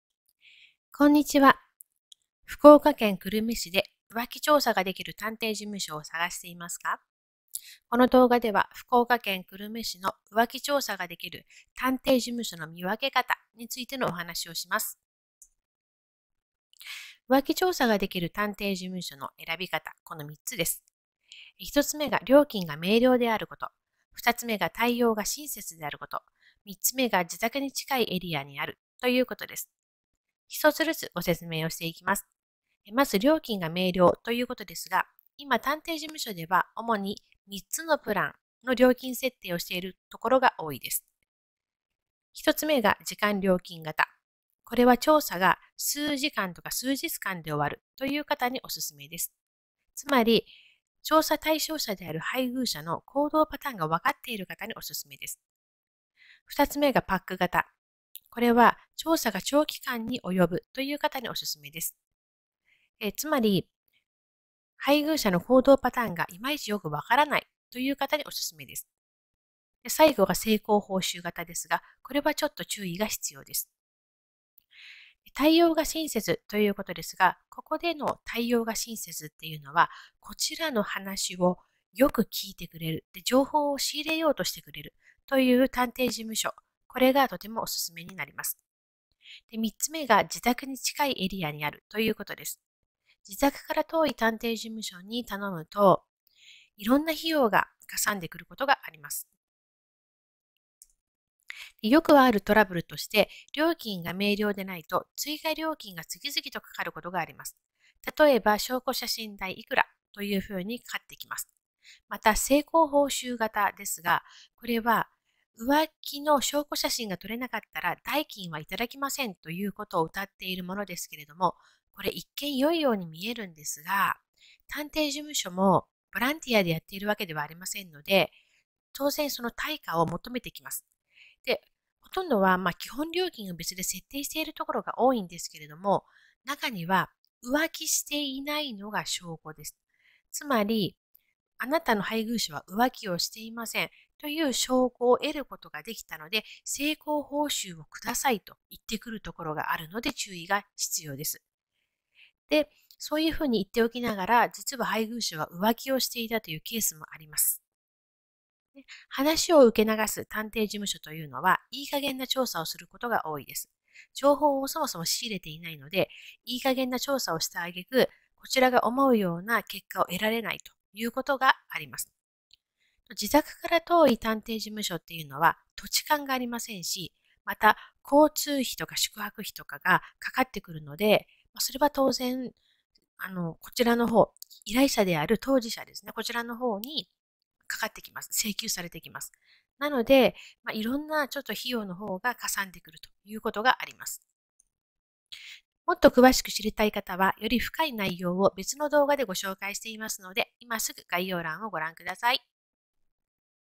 浮気調査を福岡県久留米市でしたいときの探偵事務所の見分け方 【3分でわかる】
浮気調査に強い探偵事務所を無料ですぐに教えてくれるサイト
（時間のない方はこちら）

浮気調査に優れた探偵事務所の選び方

浮気調査に強い探偵を上手に選ぶポイントを3分で紹介。
最も早く安く安心出来る探偵事務所（興信所）を選ぶ方法を教えます。
浮気調査を福岡県久留米市でしたいときの探偵事務所の選び方についてお話しています。
浮気調査をするために福岡県久留米市で探偵事務所を探しているあなた！自分の力だけで探すのはかなり大変！ですから、便利な無料サービスを使いましょう。
今すぐ動画をご覧下さいね！